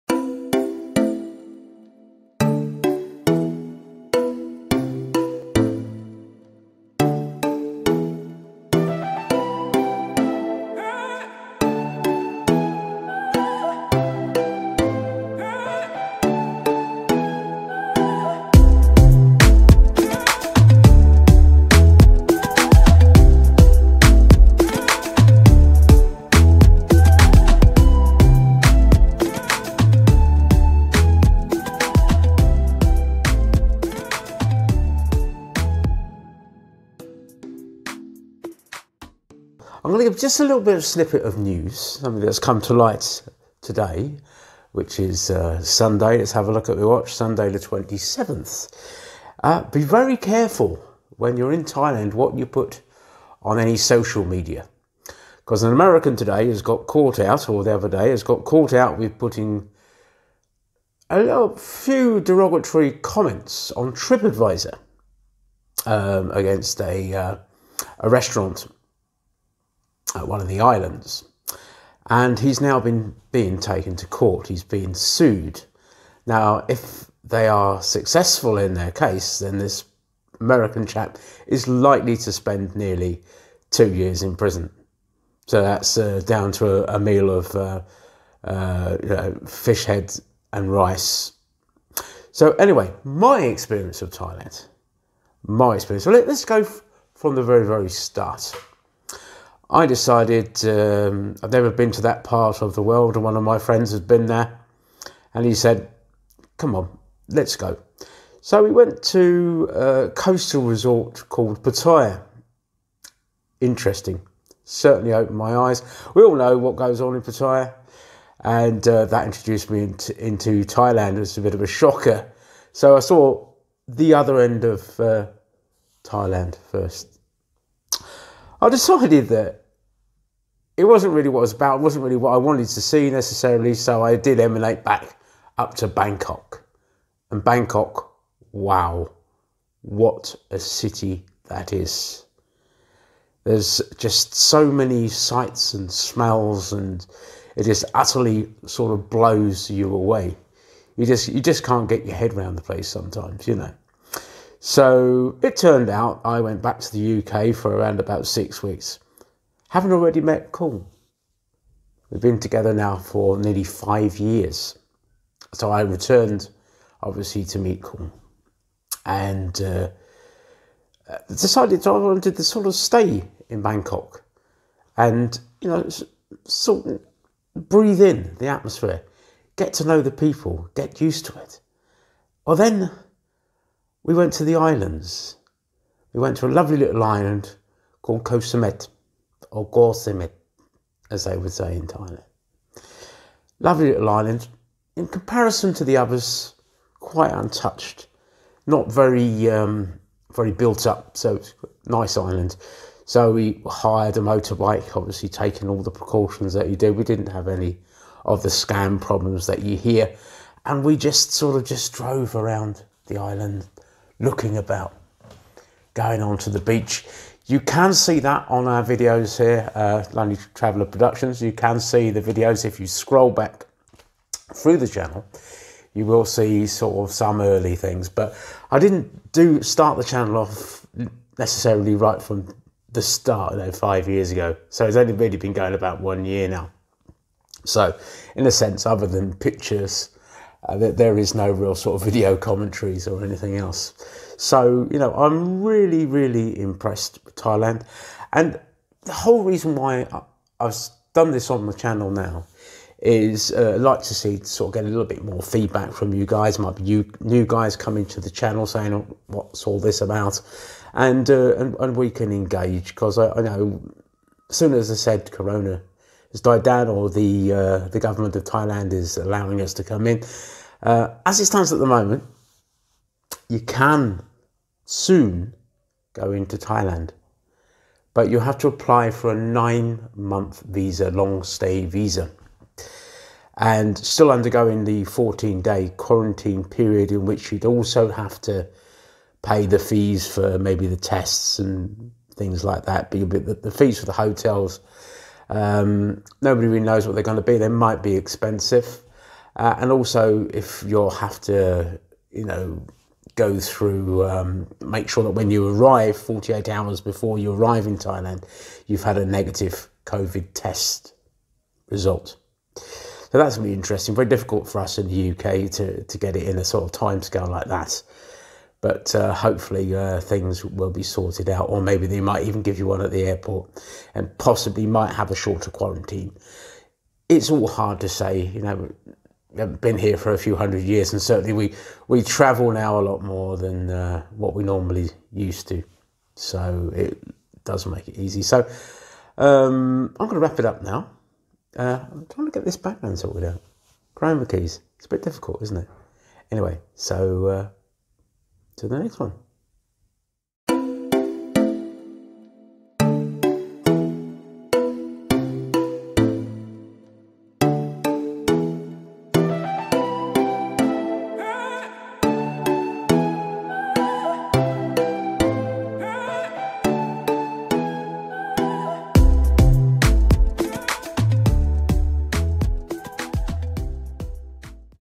All about Thailand vlog 1
New to Lonely traveler productions
All about Thailand, is an Information and story Vlog, about Thailand
Please get involved, tell me what you want to hear in the comments section below
would love to get a response
well stay safe everyone love you all